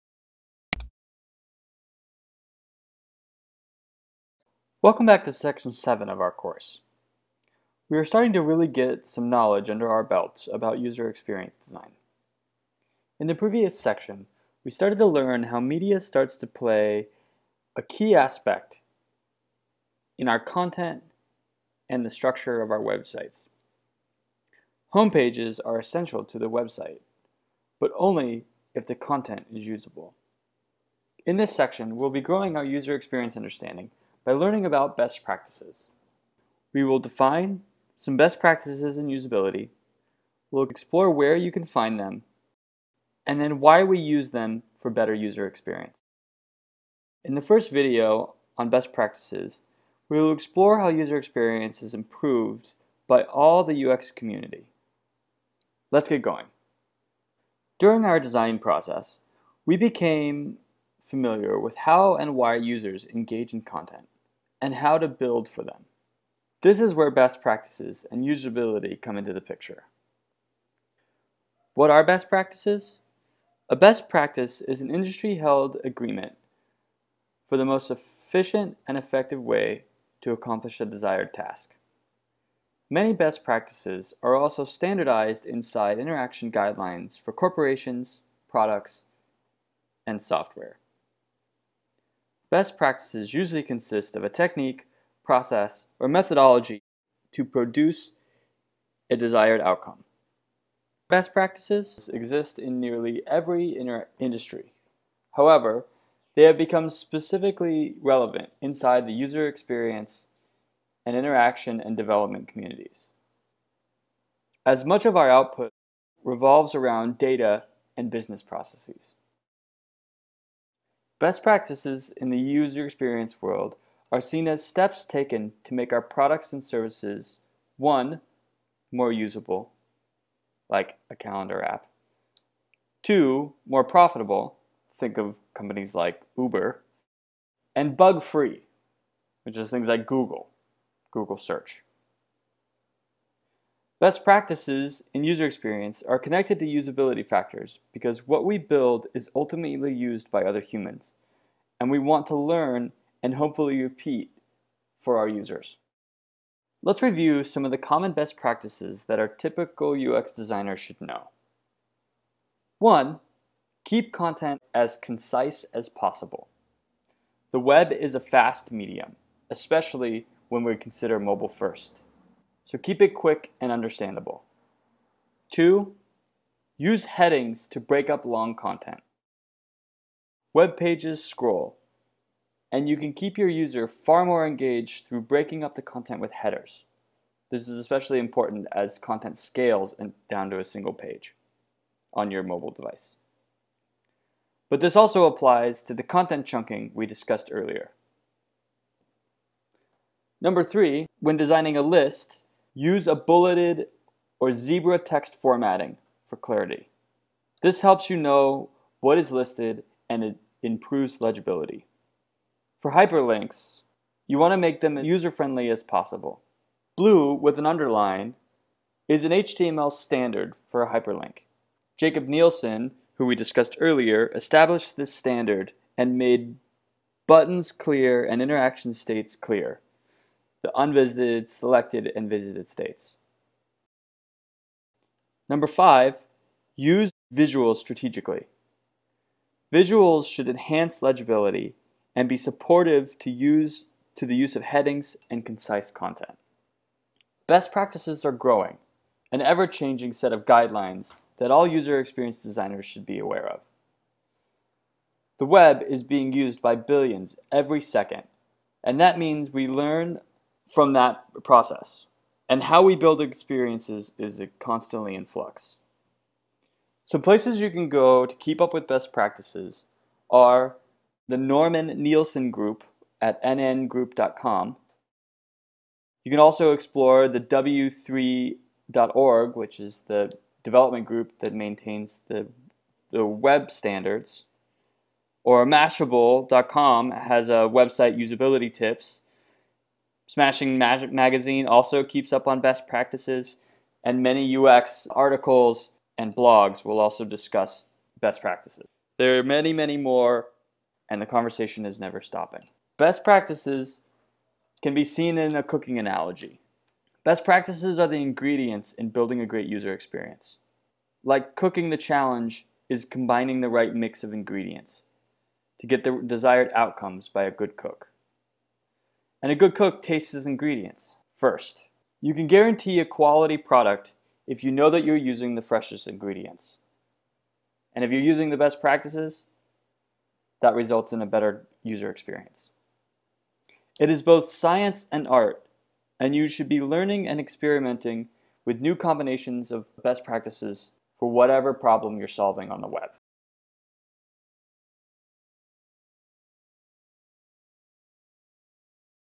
UX Design for Web Developers: Discuss Best Practices in UX Design | packtpub.com
Go through the best practices in place for UX design.
• Define best practices and usability
• Where you find best practices
• Why UX uses best practices